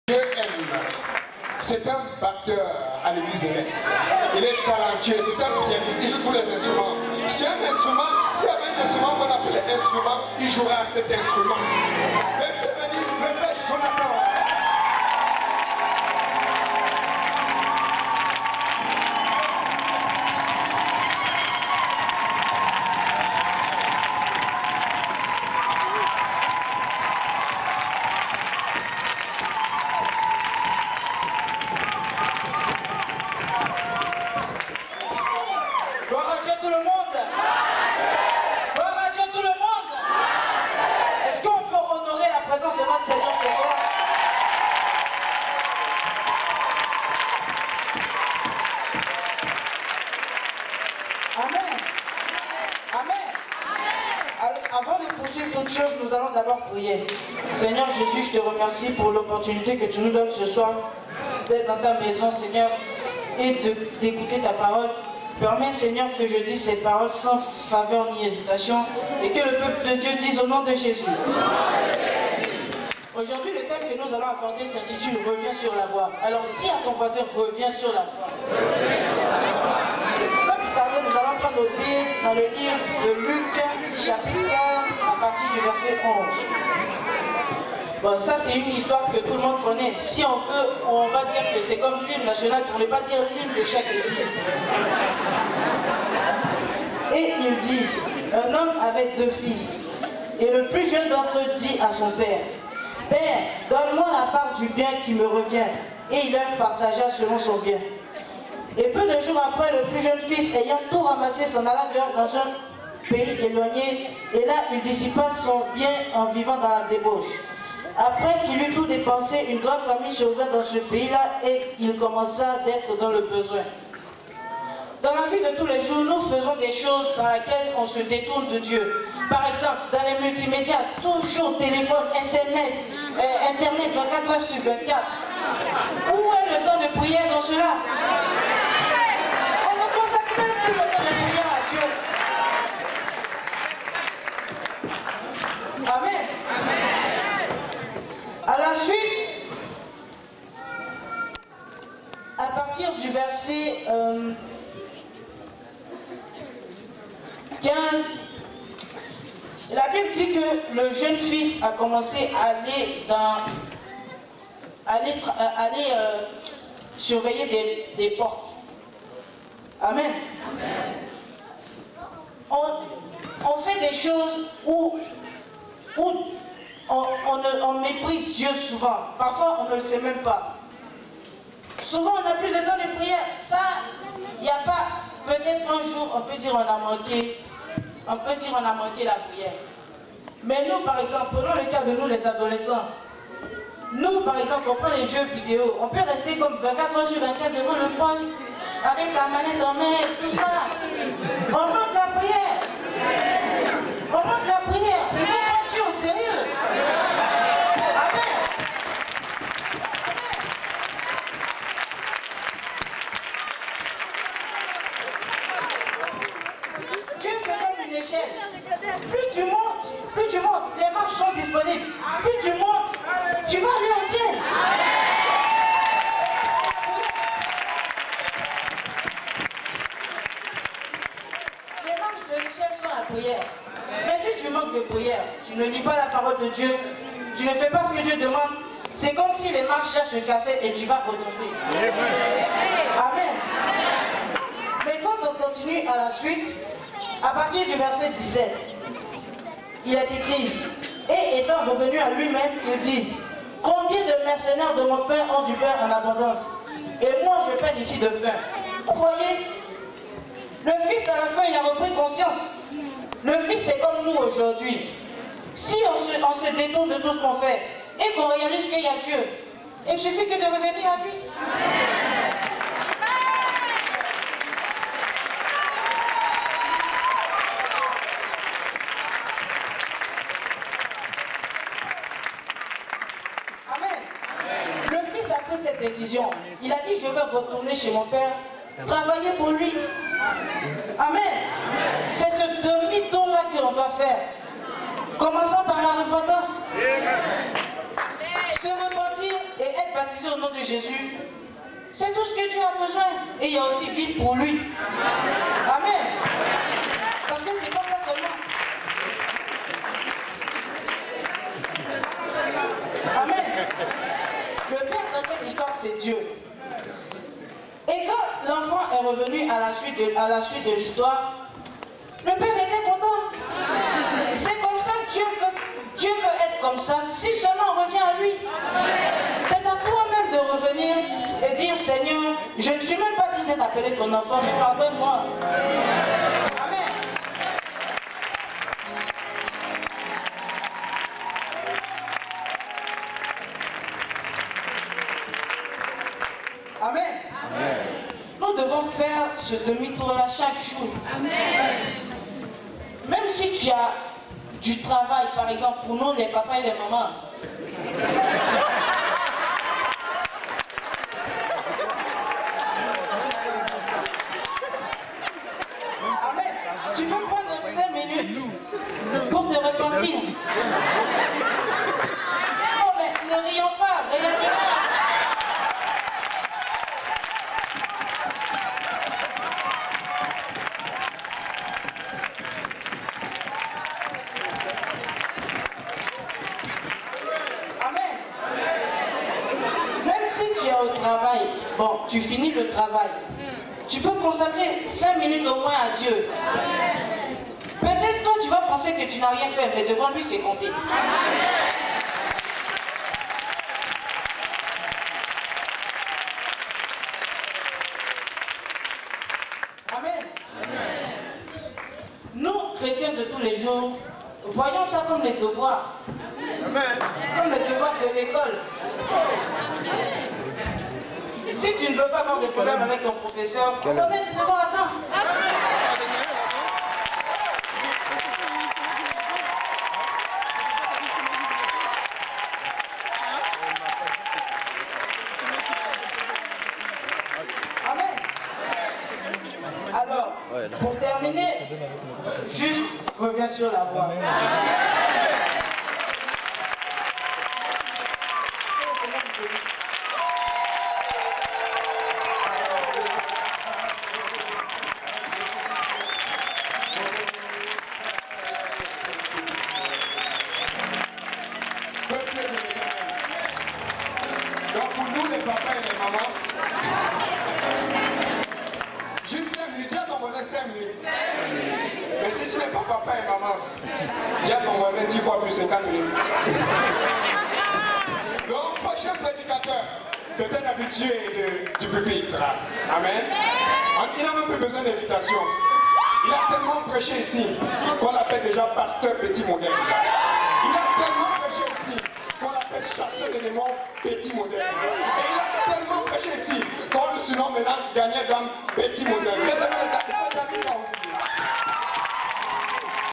CONFÉRENCE FRANCOPHONE 2018 - EXPLOSION DE LA PAROLE - FRÈRE JONATHAN TOH
SAMEDI 10 NOVEMBRE 2018 À L’ÉGLISE PENTECÔTISTE UNIE DE SAINT-LAURENT

NOS EGLISES:

          1375 POIRIER, SAINT-LAURENT, H4L 5K7

           8729 HOCHELAGA, H1L 2M8.